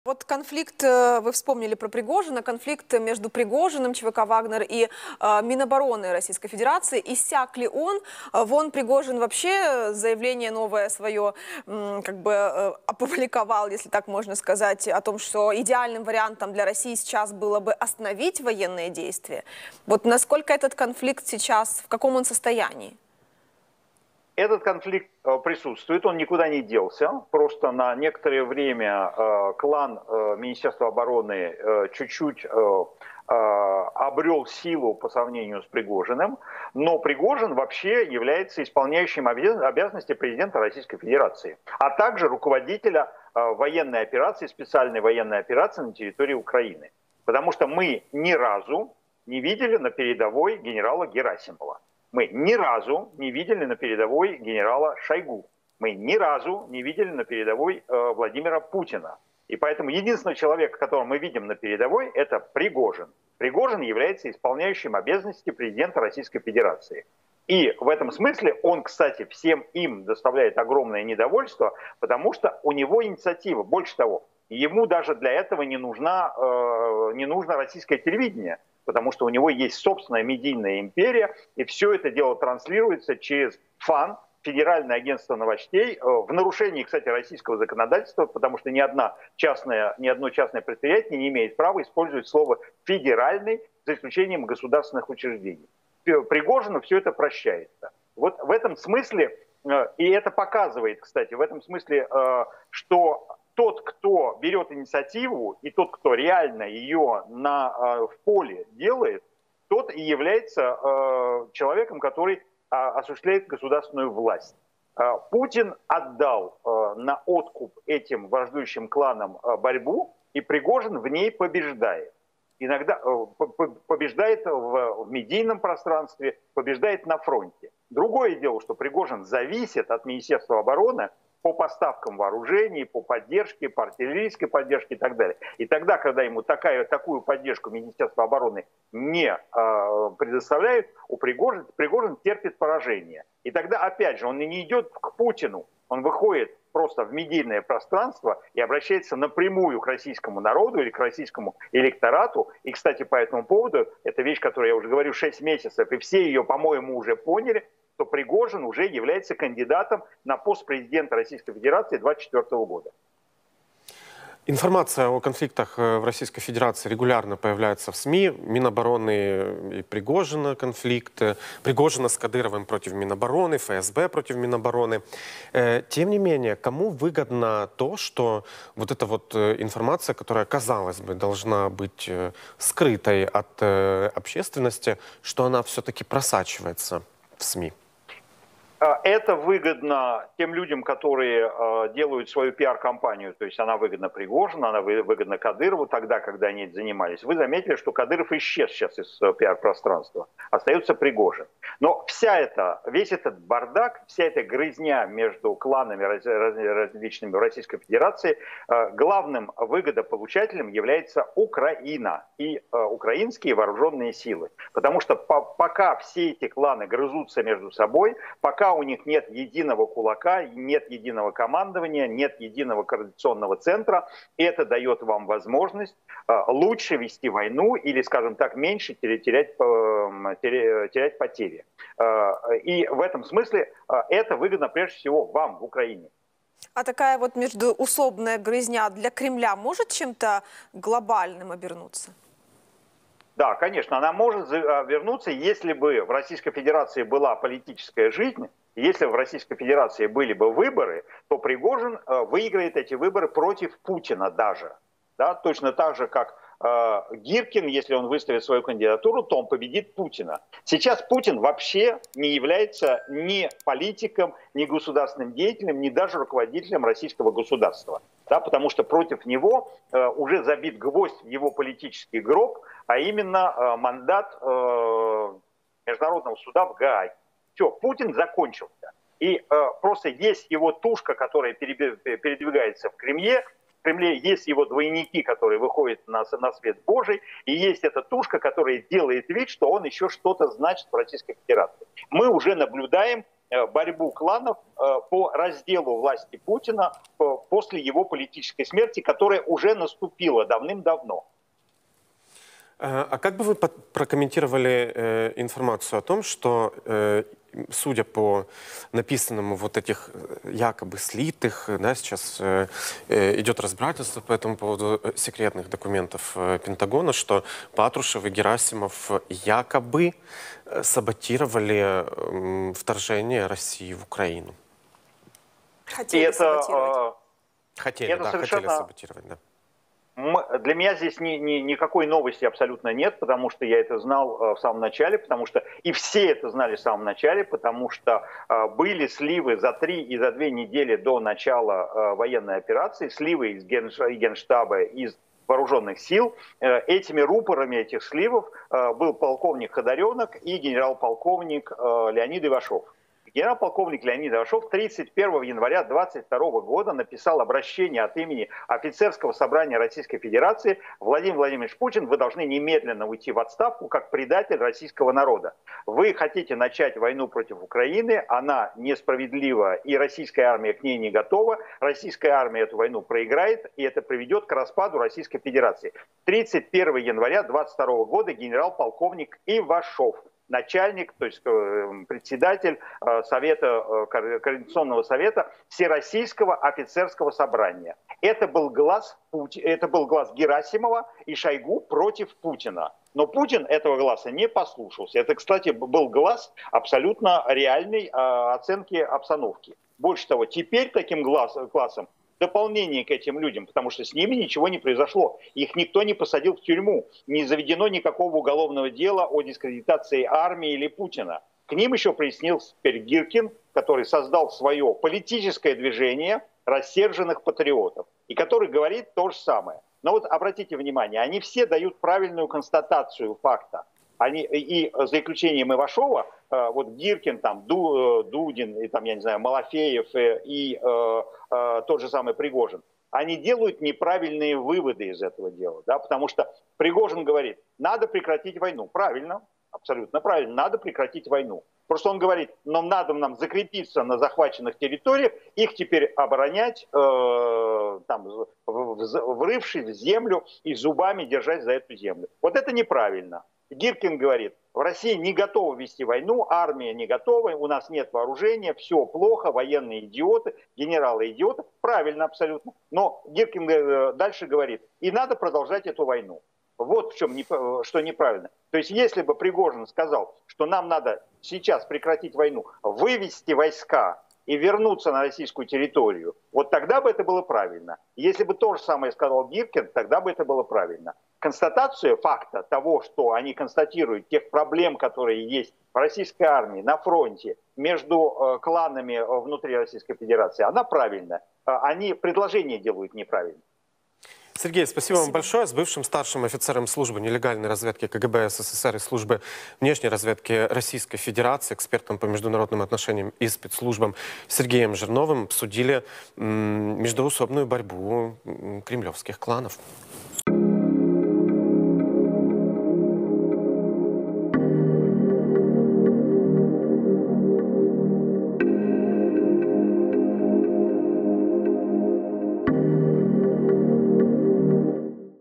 Путин - политический труп. У Володи отняли всю власть | Жирнов
RU - Владимир Путин, как политическая фигура, уже полностью закончился. Российские элиты приступили к разделу его власти. Детали озвучил офицер КГБ, бывший разведчик Сергей Жирнов.

UA - Володимир Путін, як політична фігура, вже повністю закінчився. Російські еліти розпочали поділ його влади. Деталі озвучив офіцер КДБ, колишній розвідник Сергій Жирнов.